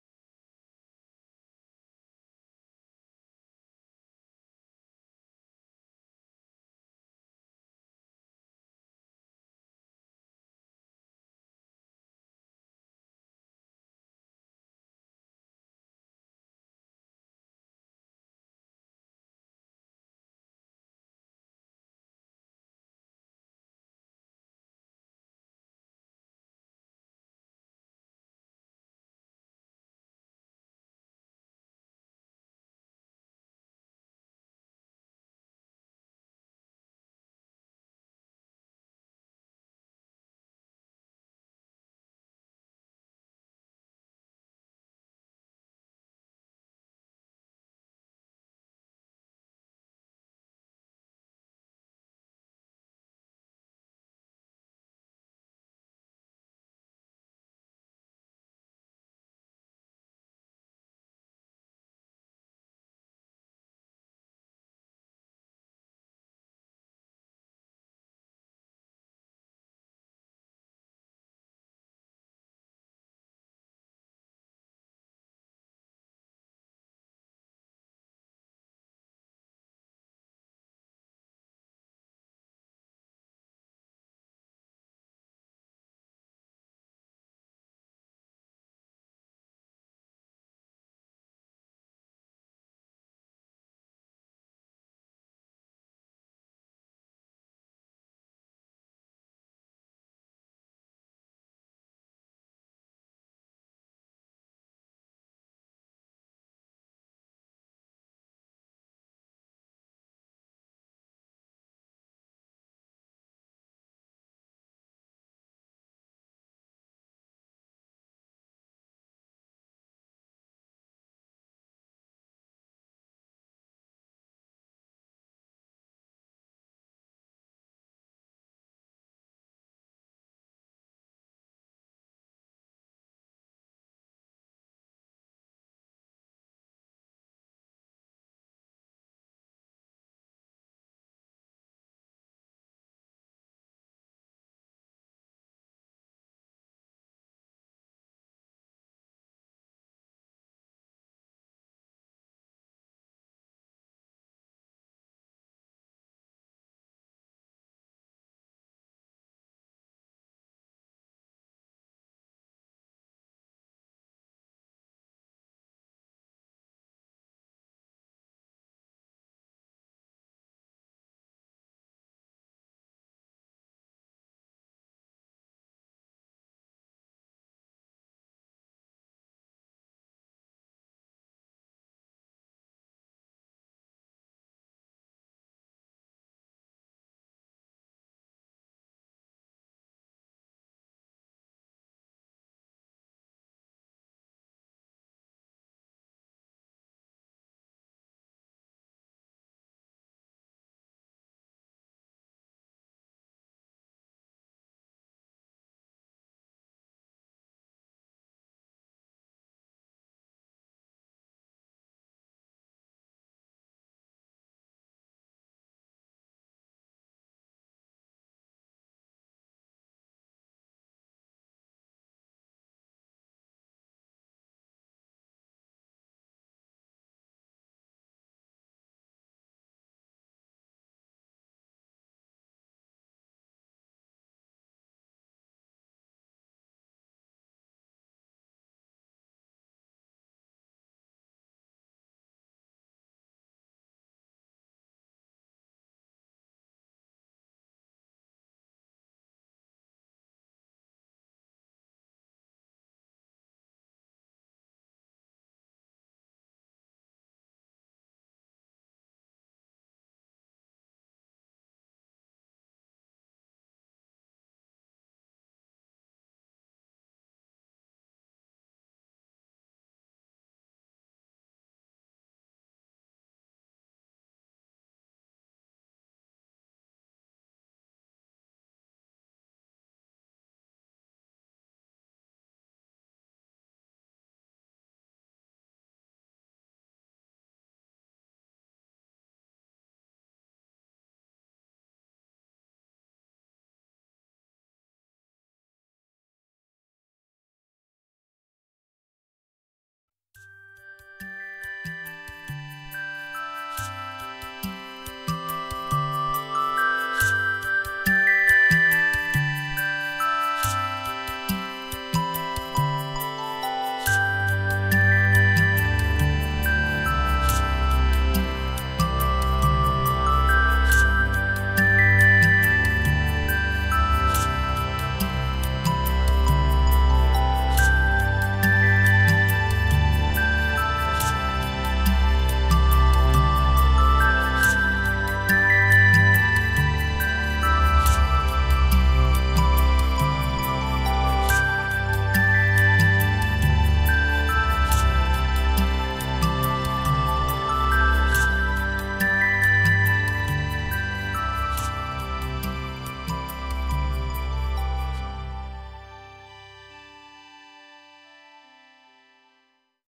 5 minutos para crianças com alarme 🎶 Diversão na sala de aula, gestão do lar e do tempo de tela 🌟
¡Bienvenidos al Temporizador de 5 minutos para niños con alarma! 🎉 Perfecto para ayudar a los niños a concentrarse en el aula, completar tareas en casa y gestionar el tiempo frente a la pantalla. Con música instrumental alegre, cada tarea se vuelve divertida. 🎶

📚 Ideal para:

Maestros: Gestionar actividades en el aula y mantener a los estudiantes enfocados 🏫
Padres: Organizar las tareas del hogar y planificar tiempos de estudio 🏡
Niños: Equilibrar el tiempo de pantalla y de juego 🎮
Disfruta de la cuenta regresiva, la música y trae estructura y diversión a tu día. 🌟